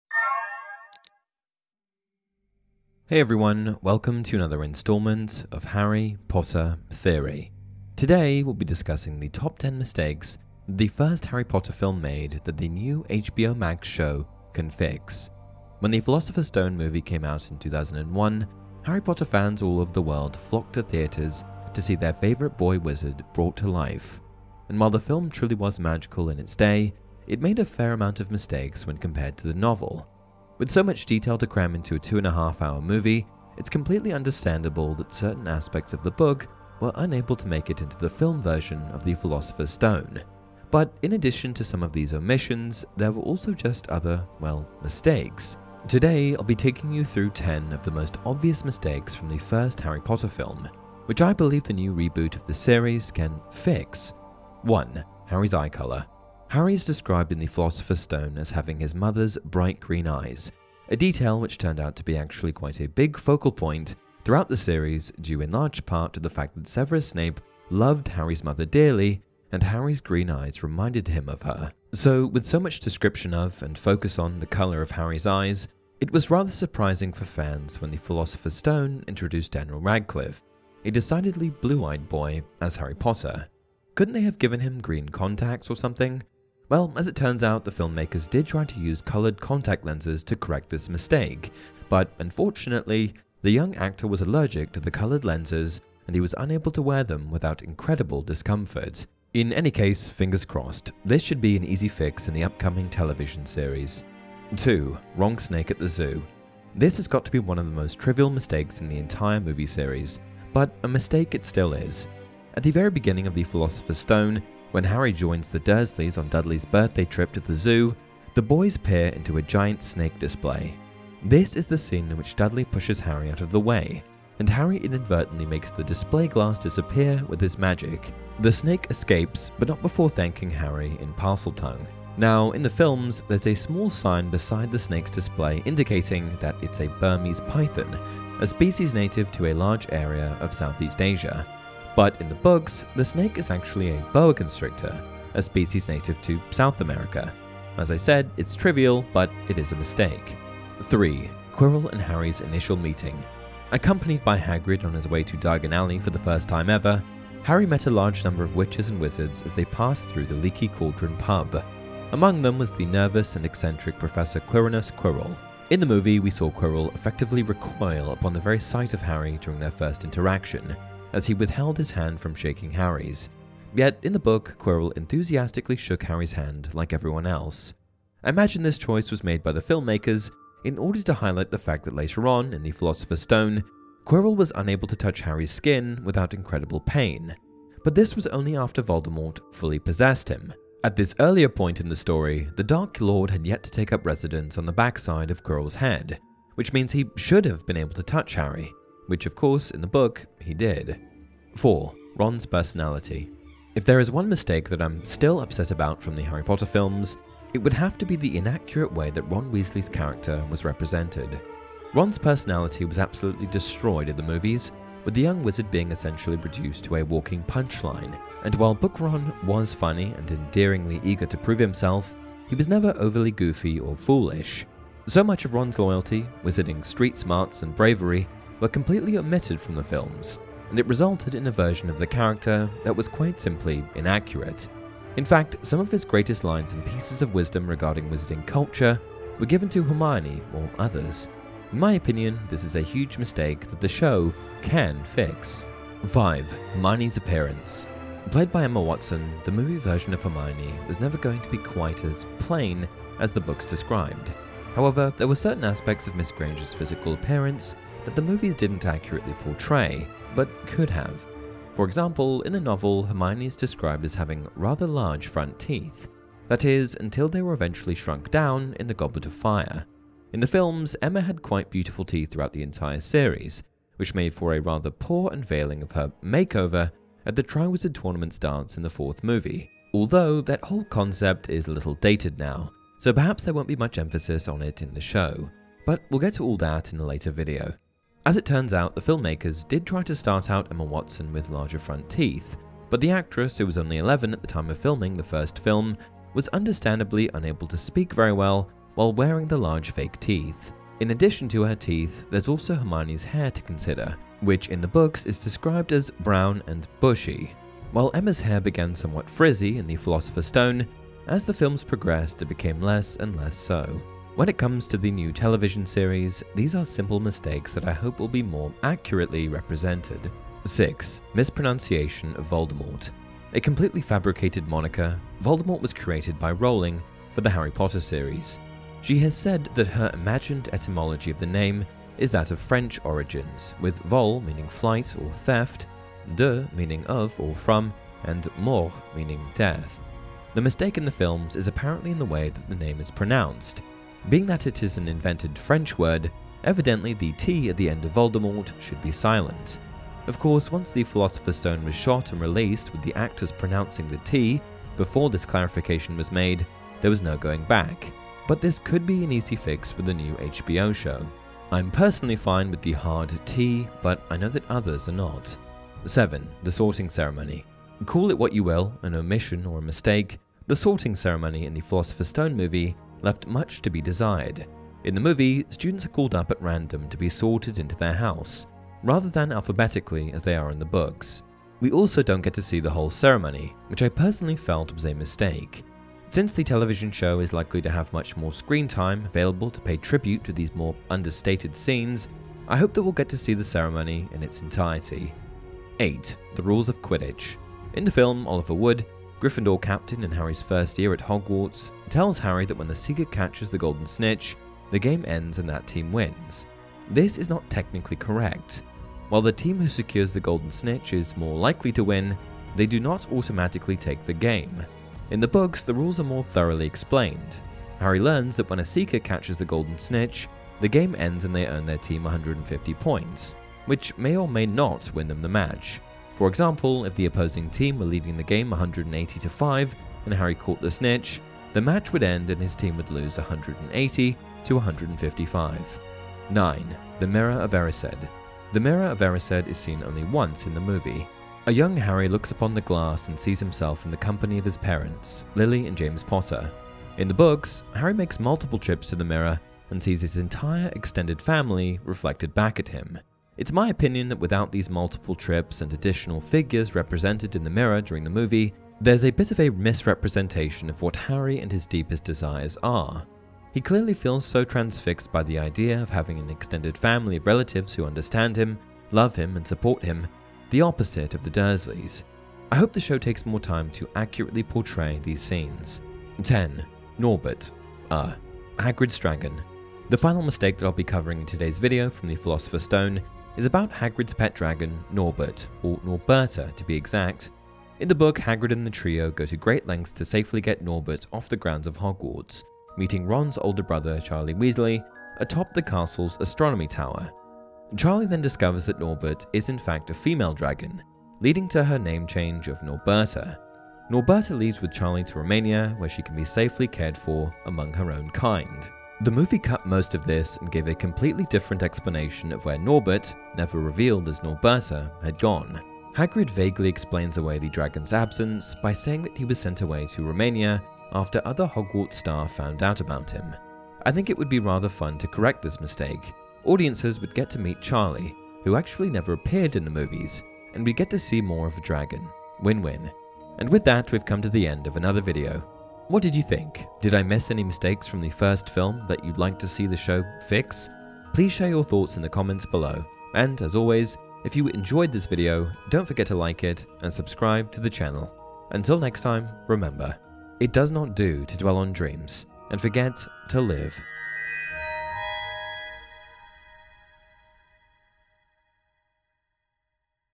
10 Philosopher's Stone Mistakes the Harry Potter Reboot Can FIX - Harry Potter Explained
Welcome to Harry Potter Theory. Today we're discussing the top 10 mistakes that the first Harry Potter film made that the new HBO Max SHOW can FIX.

When The Philosopher’s Stone movie came out in 2001, Harry Potter fans all over the world flocked to theatres to see their favourite boy wizard brought to life. And while the film truly was magical in its day, it made a fair amount of mistakes when compared to the novel.

With so much detail to cram into a two-and-a-half-hour movie, it’s completely understandable that certain aspects of the book were unable to make it into the film version of The Philosopher’s Stone. But in addition to some of these omissions, there were also just other, well, MISTAKES.

Today, I’ll be taking you through 10 of the most obvious mistakes from the first Harry Potter film, which I believe the new reboot of the series can FIX.

▼MAILING ADDRESS
HP Theory
North Vancouver RPO The Quay
BC
V7M 0G4

Thanks!